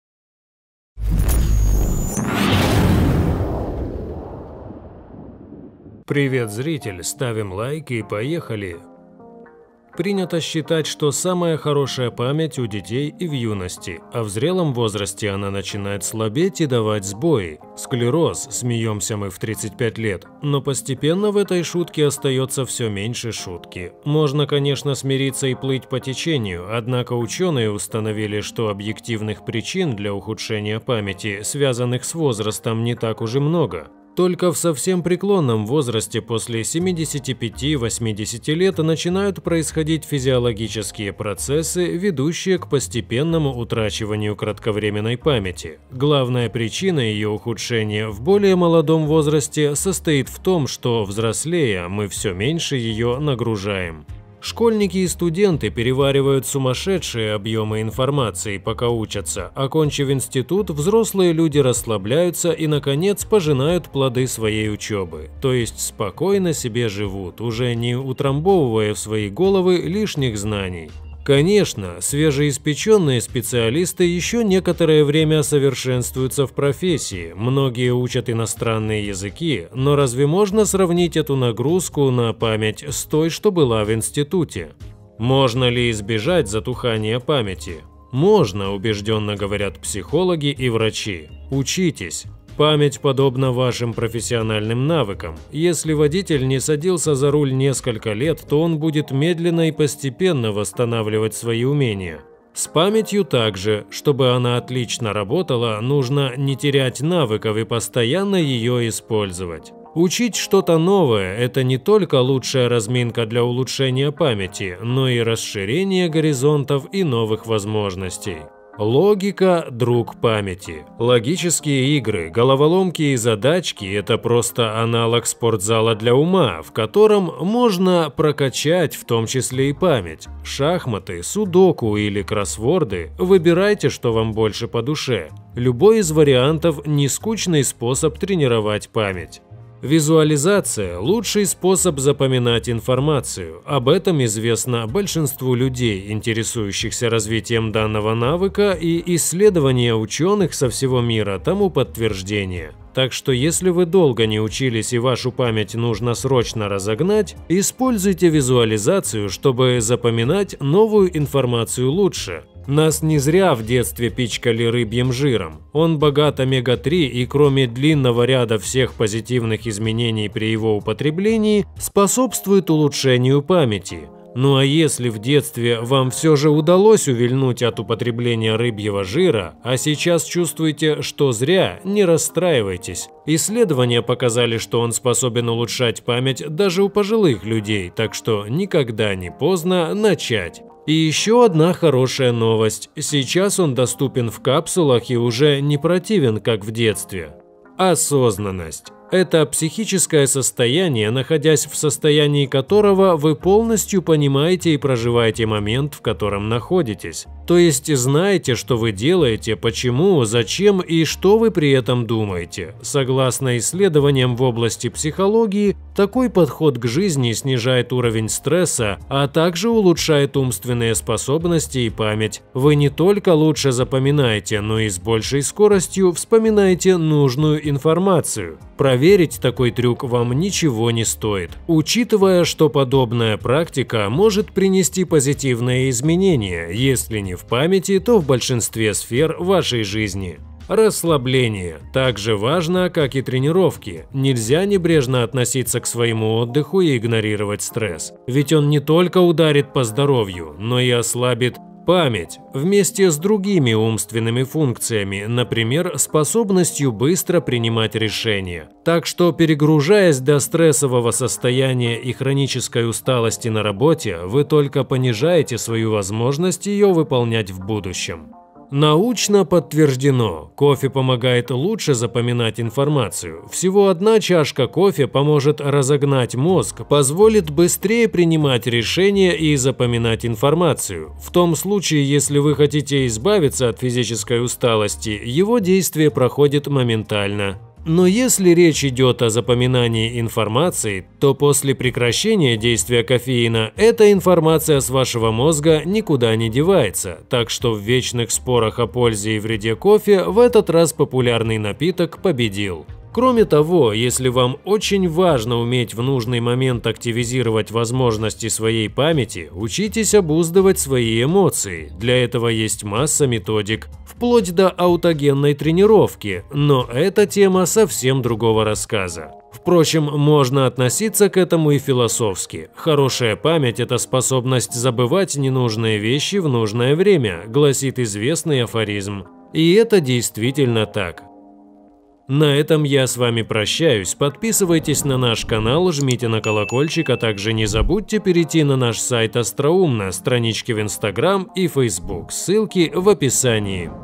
7 трюков по улучшению памяти, которые могут помочь
Принято считать, что самая хорошая память – у детей и в юности. А в зрелом возрасте она начинает слабеть и давать сбои. «Склероз» – смеемся мы в 35 лет, но постепенно в этой шутке остается все меньше шутки. В этом ролике, мы расскажем Вам, как можно чуток помочь своей памяти!